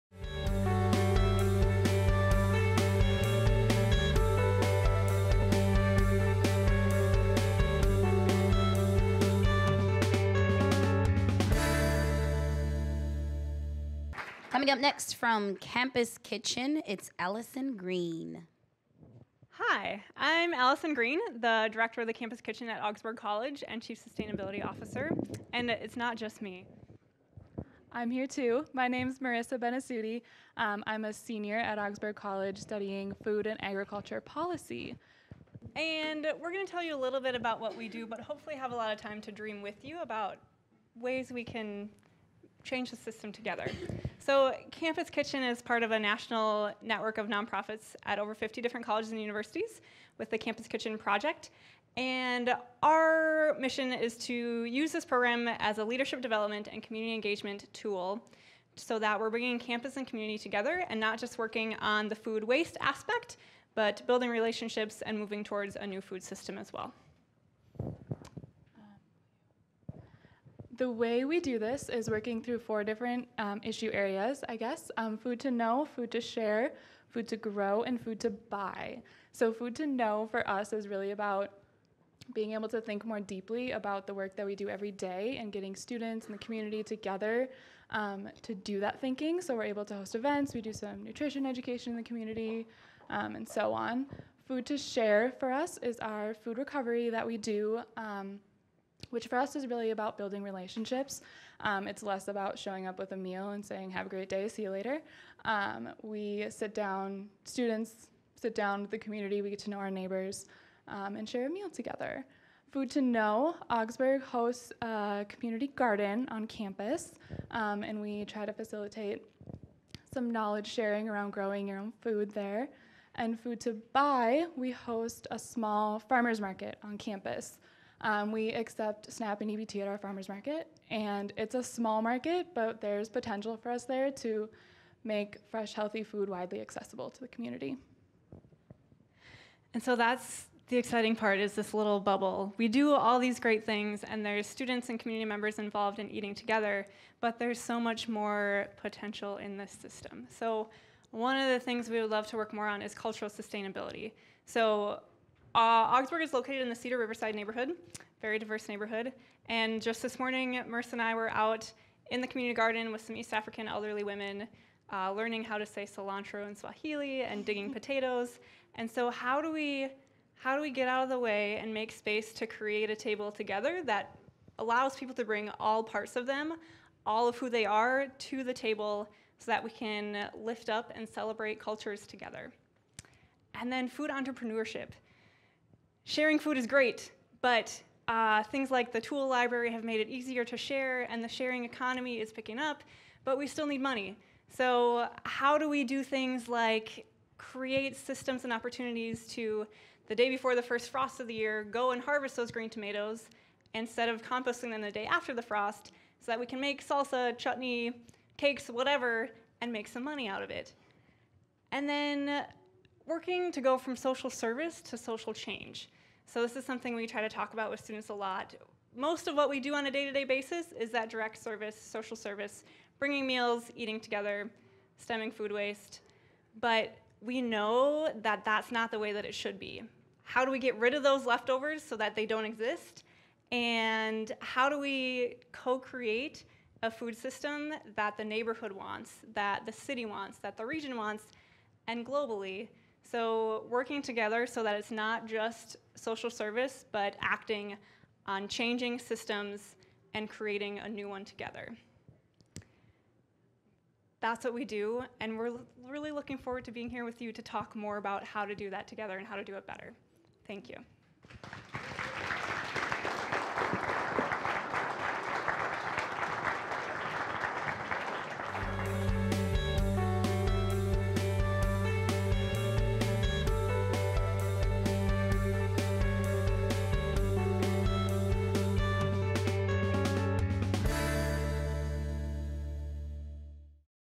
Allyson Green and Marisa Benasutti, Campus Kitchen - ZERO WASTE SUMMIT 2016
Allyson Green – Director
Allyson joined the Campus Kitchen in Fall 2014 after completing an MS in Environmental Justice and Environmental Health. While in graduate school, she spent her time organizing students and digging in the dirt with the University of Michigan Sustainable Food Program and Campus Farm. Her previous experience includes public health research on small-scale gold mining in Ghana, environmental education in Milwaukee, community outreach on water quality and coal mining in Appalachia, and science education research and service learning at Calvin College. She works with food because everyone eats food – it’s a common ground issue that is connected to myriad complex environmental and social issues. When she’s not serving meals or gardening with students, she can be found canning, making homemade ice cream, or wandering in the woods with a Wendell Berry book in hand.

Marisa Benasutti
Senior, Major in Food Policy and Sustainable Agriculture, French Minor

On October 22, 2016 Eureka Recycling hosted the second annual Zero Waste Summit at the Saint Paul Neighborhood Network. Local and national speakers shared their work and vision for zero waste and made many important connections with attendees who are also working to bring equitable solutions to the problems associated with waste to their own communities.